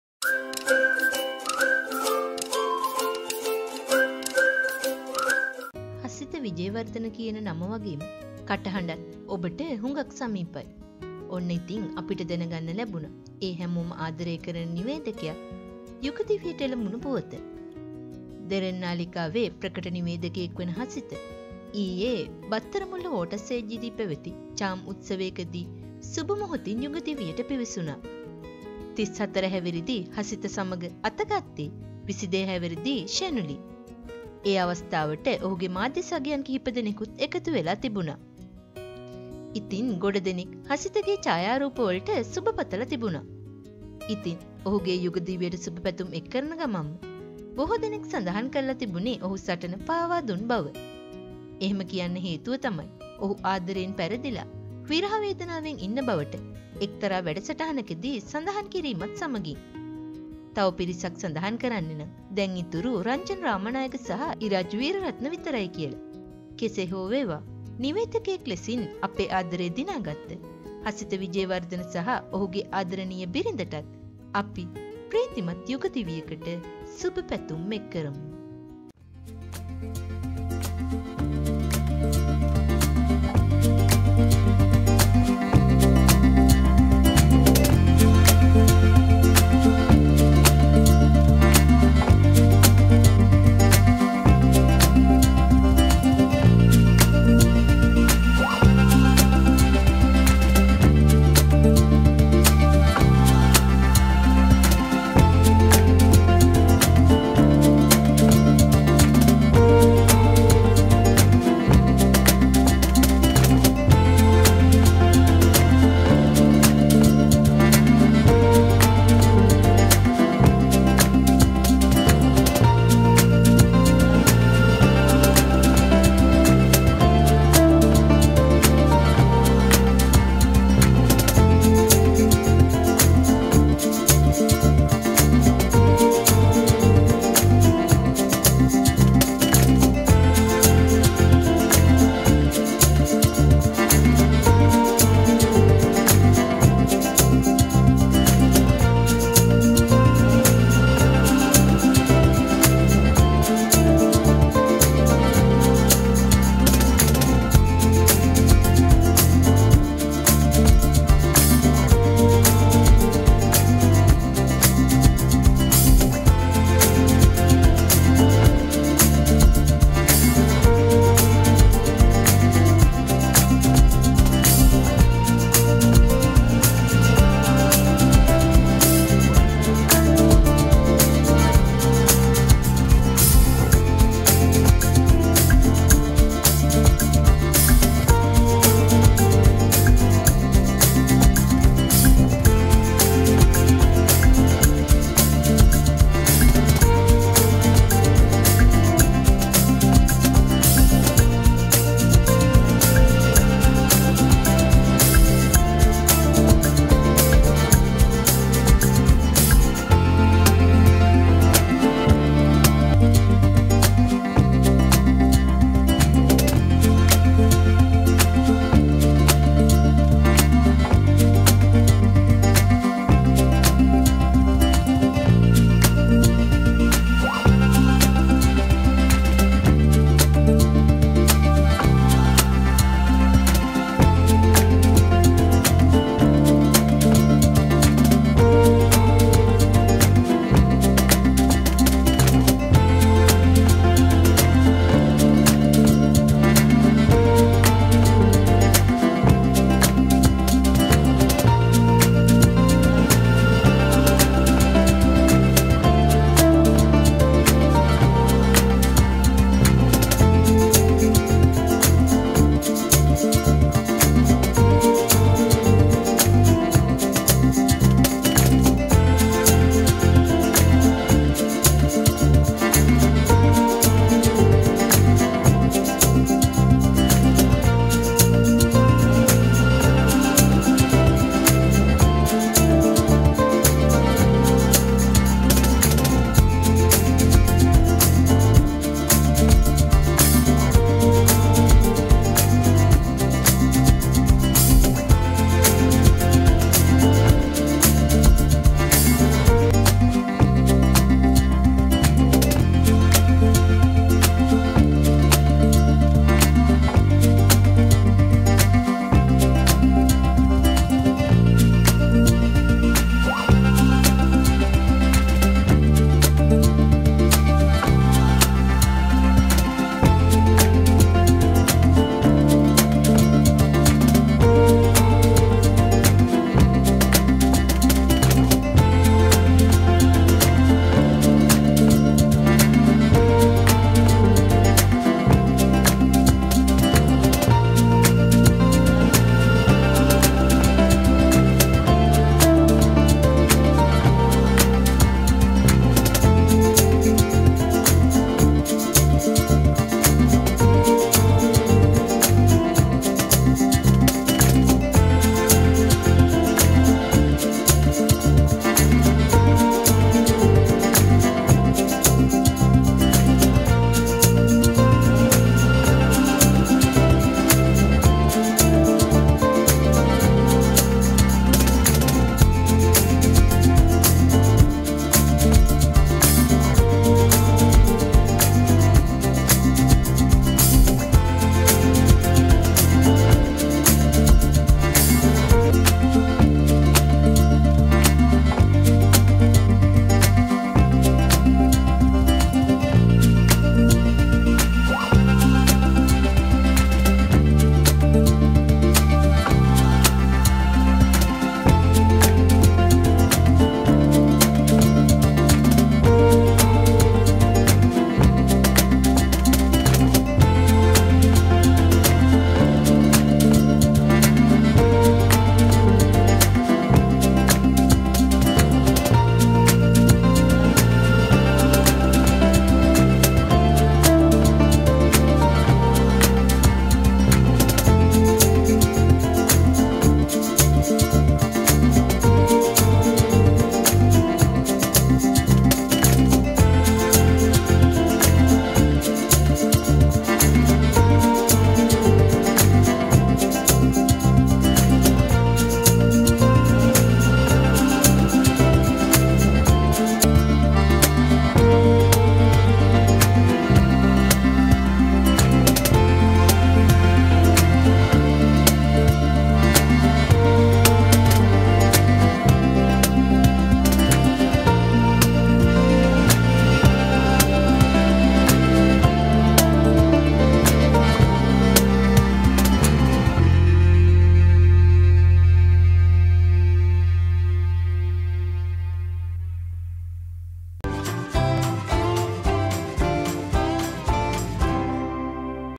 Hasitha Wijewardana & Shenuli wedding සටන පාවා දෙමින් ජනප්‍රිය නිවේදක හසිත සහ ශෙනුලි යුග දිවි අරඹයි
Hasitha & Liyena’s  love journey is a beautiful testament to devotion and shared dreams. From heartfelt moments to unforgettable adventures, their bond grows stronger each day. Here's to celebrating their story and wishing them a lifetime filled with joy, laughter, and everlasting love.